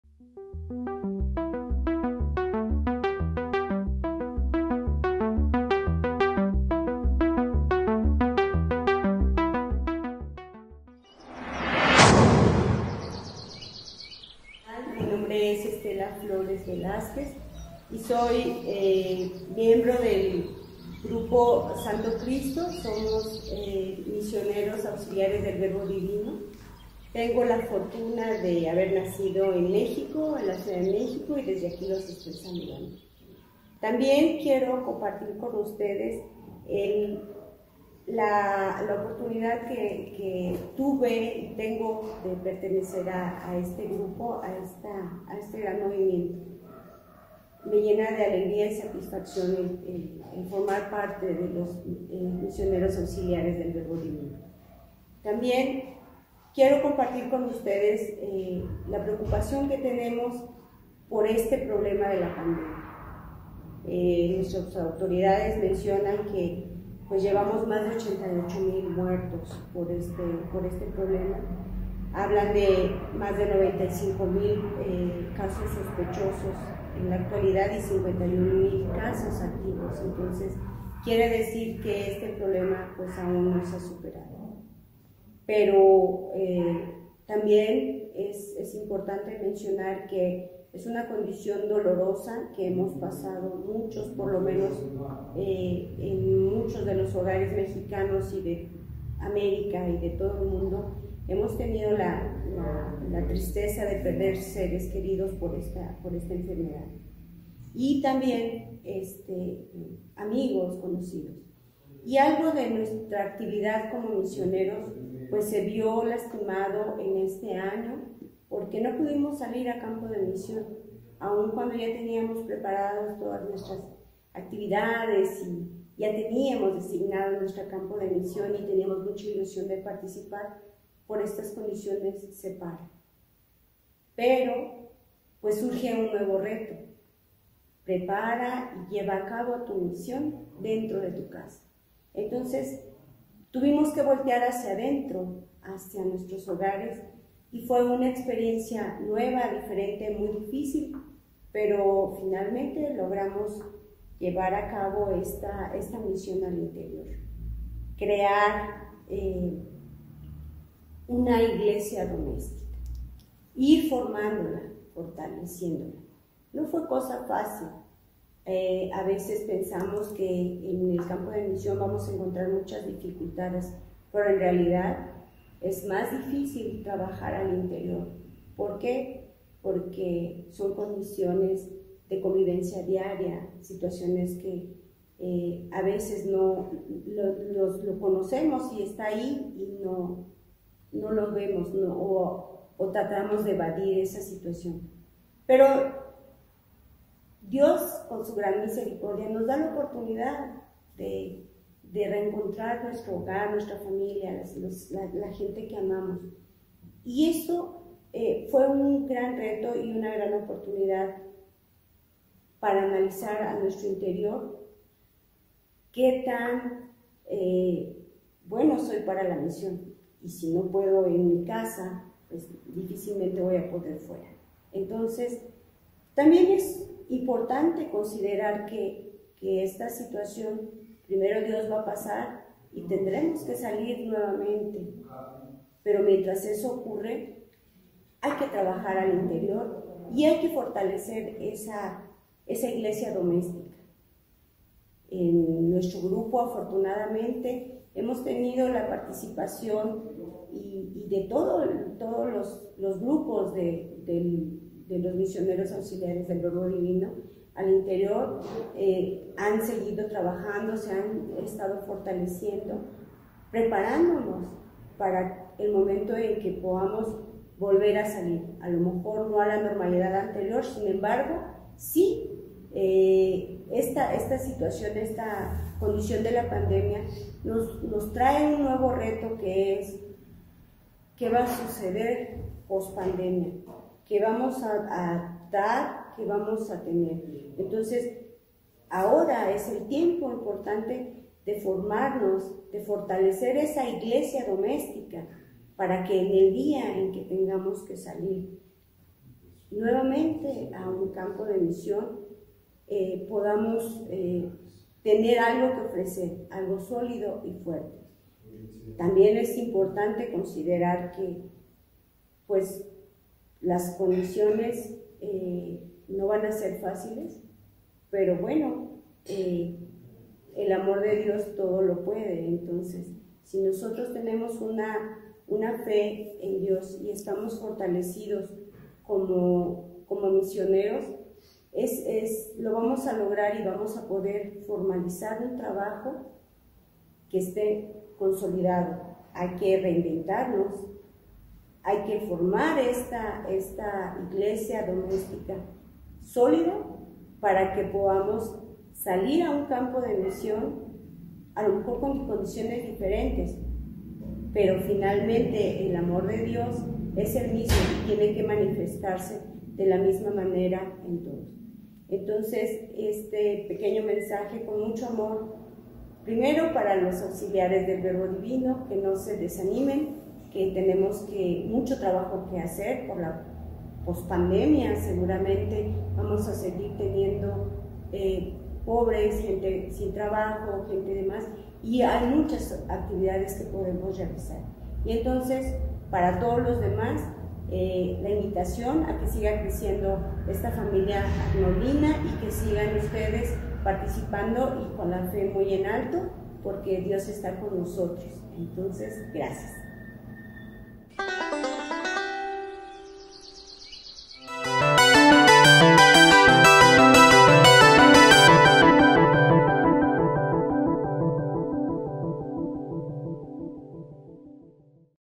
Reflexión sobre la Misión 11, MAVD y la Pandemia
Reflexión sobre la Misión 11,  Señora Estela Flores Velazquez, del grupo Sto Cristo del MAVD Mexico comparte sus pensamientos sobre la realidad de MAVD en la pandemia